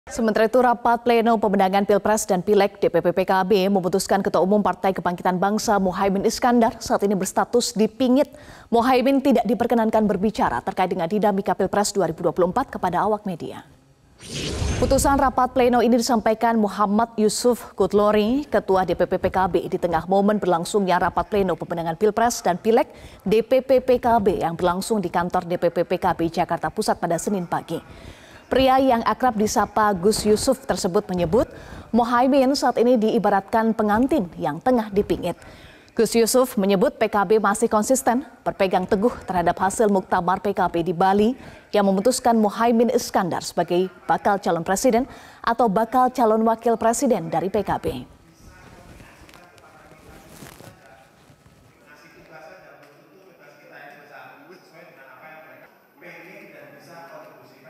Muhaimin Iskandar Berstatus "Dipingit" Jelang Pemilu 2024
MetroTV,